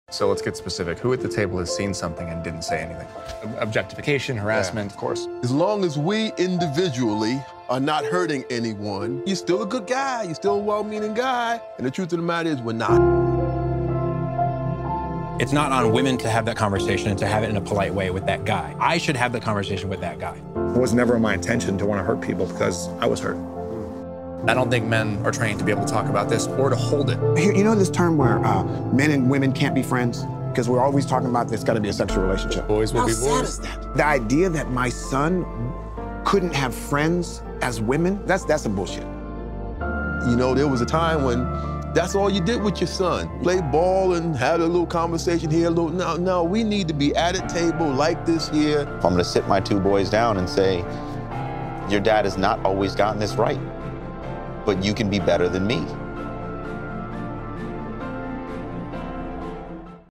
An honest conversation about men and MeToo featuring Justin Baldoni, Matt McGorry and Tony Porter.
An honest conversation about men and MeToo featuring Justin Baldoni, Matt McGorry and A CALL TO MEN CEO Tony Porter.